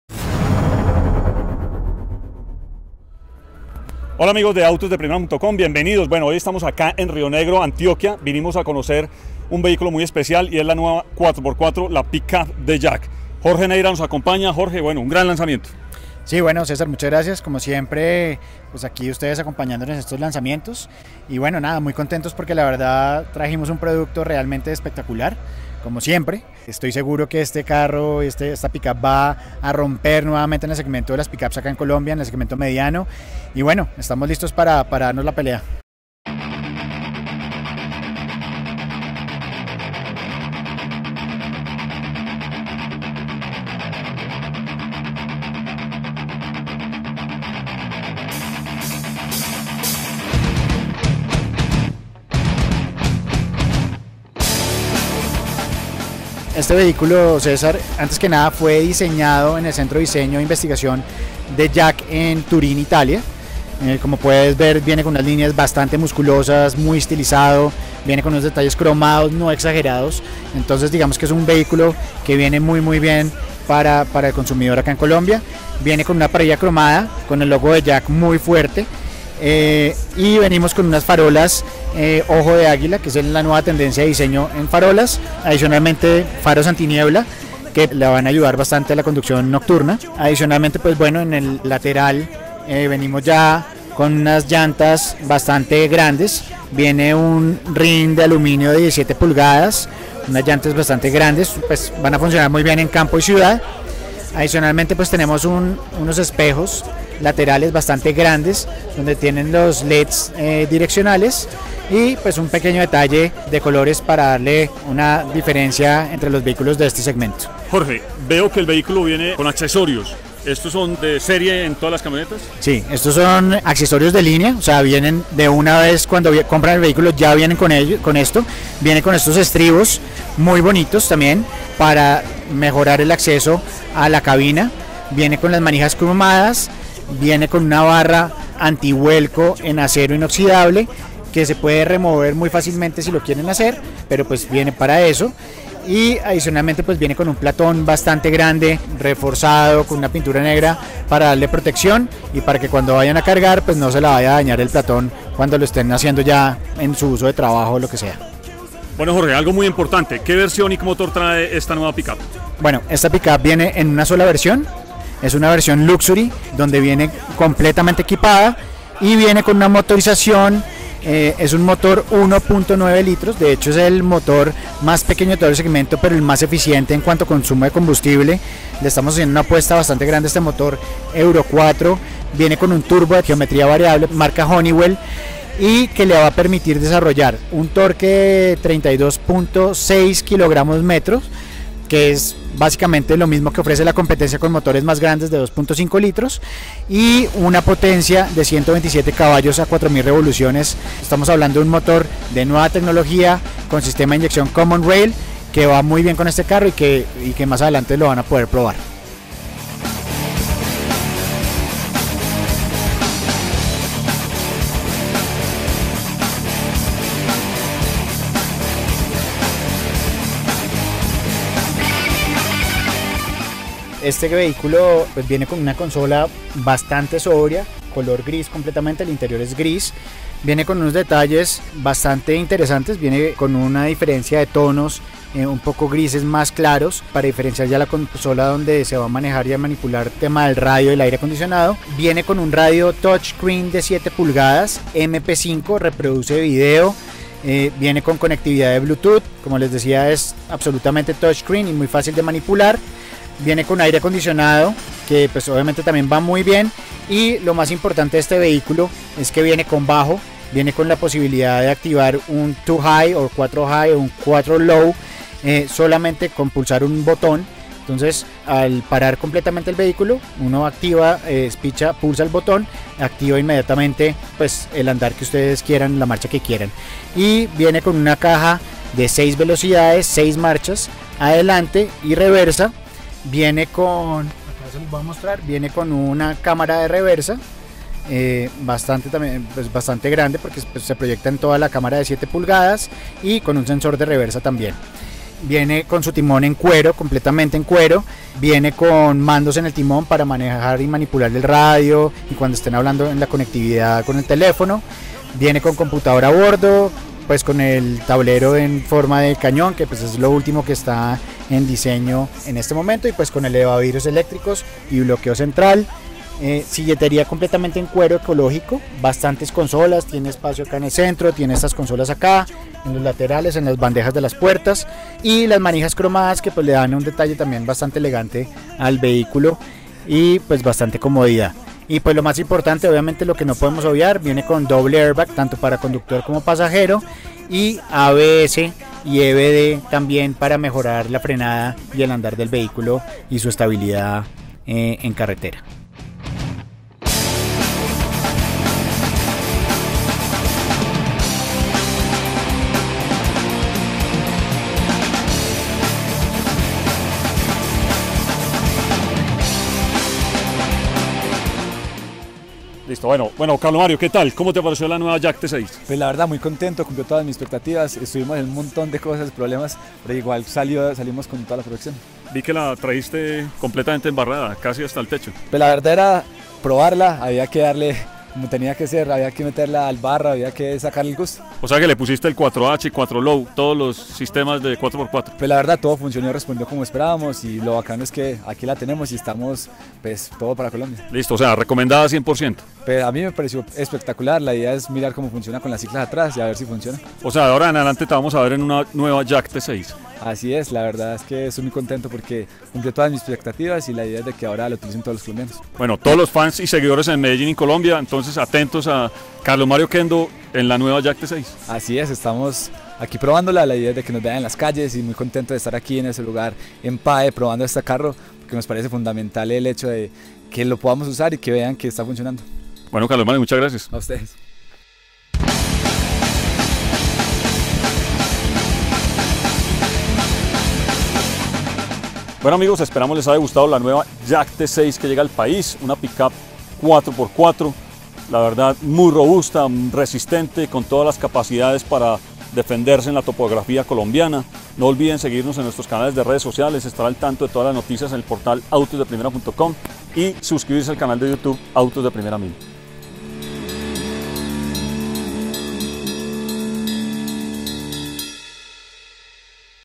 Nueva pick-up 4x4 JAC T6 en Colombia - Prueba en el Parque Aventura Escobero, Envigado
Medellín y más exactamente el PAE, Parque Aventura Escobero en Envigado, fue el escenario elegido por Autocom S.A. (representante de JAC en Colombia), para presentar y poner a prueba en difíciles condiciones off-road la nueva camioneta doble cabina "T6".